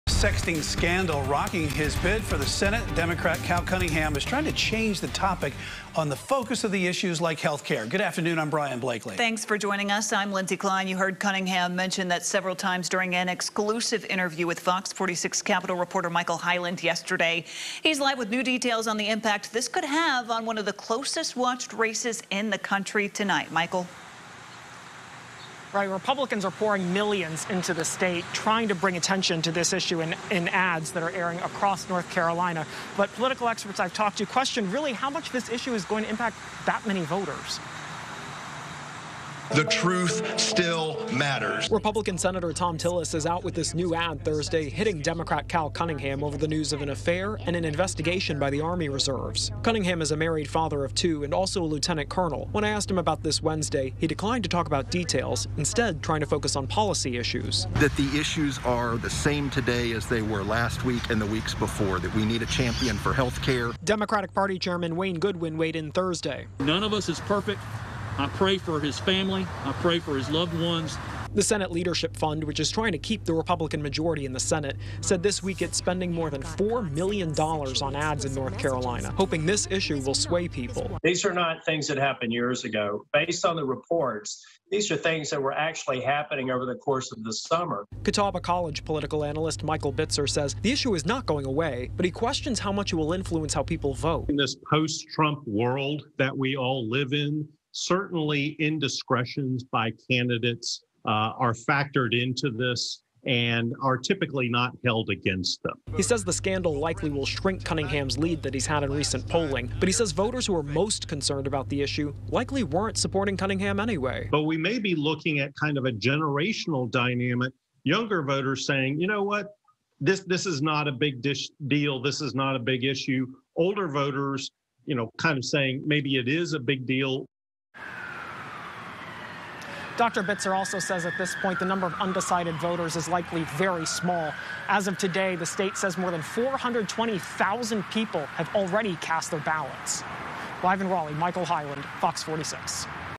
Tillis ad attacks Cunningham on 'sexting' scandal; Cunningham tries to pivot toward voter issues
With a sexting scandal rocking his bid for the Senate, Democrat Cal Cunningham is trying to change the topic to focus on issues like healthcare.